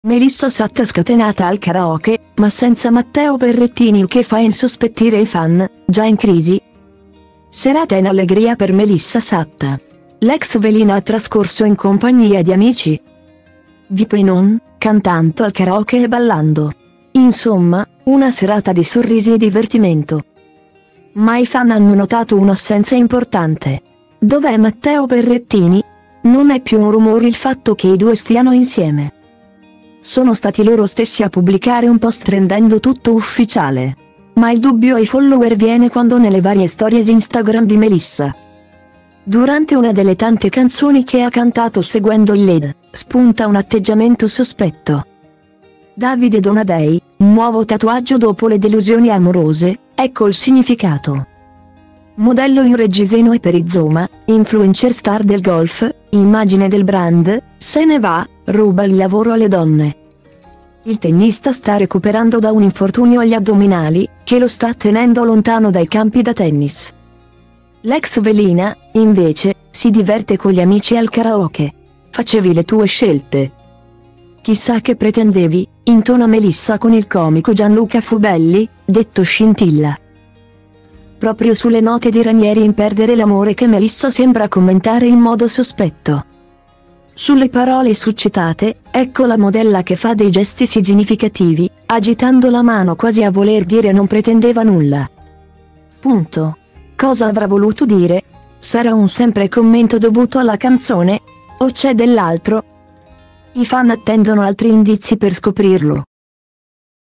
Melissa Satta scatenata al karaoke, ma senza Matteo Berrettini.Il che fa insospettire i fan: «Già i
Melissa Satta scatenata al karaoke, ma senza Matteo Berrettini.Il  che fa insospettire i fan: «Già in crisi?»
Serata in allegria per Melissa Satta. L'ex velina ha trascorso in compagnia di amici,vip e non, cantanto al karaoke e ballando. Insomma, una serata di sorrisi e divertimento,ma i fan hanno notato un'assenza importante. Dov'è Matteo Berrettini? Non è più un rumor il fatto che i due stiano insieme,